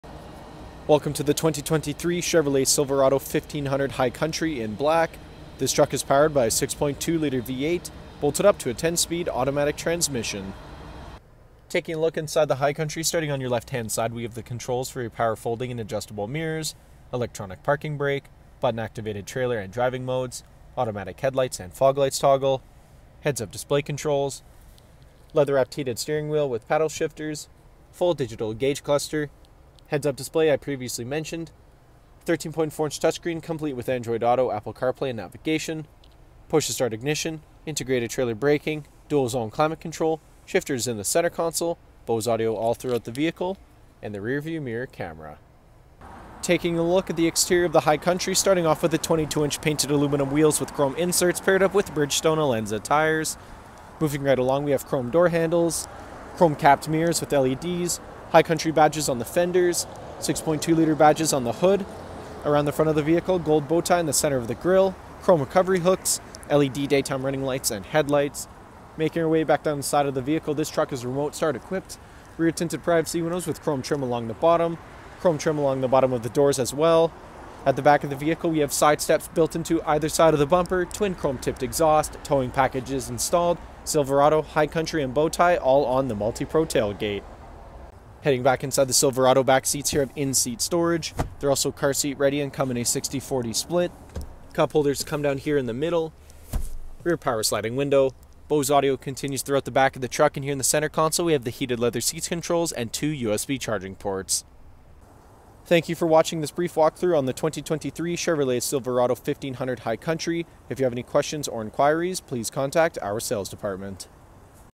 Black 2023 Chevrolet Silverado 1500 High Country Review Calgary AB - Wolfe Calgary
The New Chevrolet 1500 Silverado High Country is sure to enhance your daily drive. This model comes with a 6.2L V8 engine with 4-wheel drive and a 10-speed automatic transmission. High Country comes with safety in mind with desirable safety features, and an attractive exterior. High Country has projection style lighting with LED signal and daytime running lights. The side sports alloy High Country edition wheels, mirror caps and door handles, plus a set of running boards. This Crew cab has a step style rear bumper, a locking tailgate with rear view camera, and the tow package to make driving practical again. Inside is where you will enjoy a premium leather seating arrangement for 5 passengers. The driver seat features heated seat control, easy exit memory seat, power windows, power mirrors, power locks and a heated steering wheel. The touchscreen display offers navigation, optional XM radio, and the outside temperature display. Lane keeping system and power pedals also help in the comfort and safety. This model has everything you desire in your next truck. Stop in today to see why we are your preferred dealer; we will earn your business!